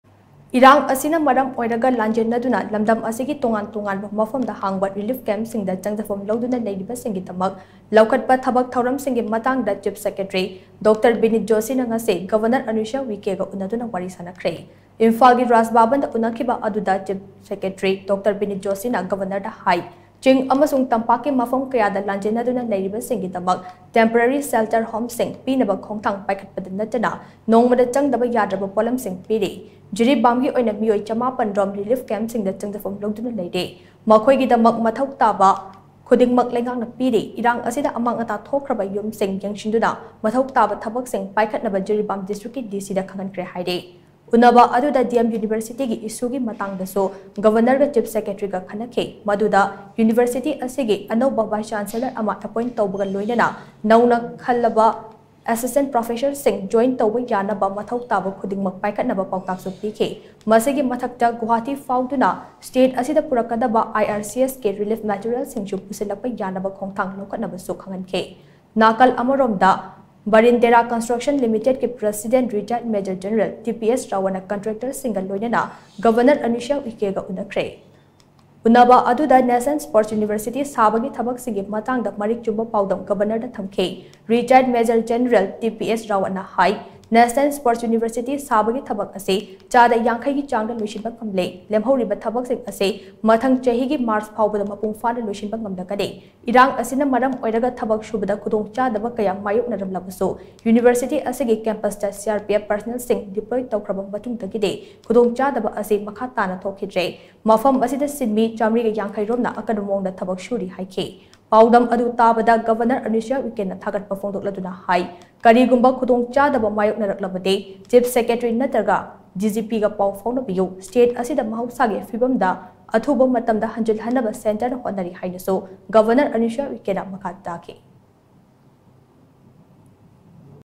RELIEF CAMP LEIRIBA SINGI MATANGDA CHIEF SECRETARY NA NGASI GOVERNOR ANUSUIYA UIKEY GA UNAKHRE
Irang asina maram oiraga lanjennaduna lamdam asigi tongan tonganba mapham da hangba relief camp shing da changjapham louduna leiriba shing gi damak loukhatpa thabak thouram shing gi matang da Chief Secretary Dr Vineet Joshi na ngasi Governor Anusuiya Uikey ga unnaduna wari shanakhre.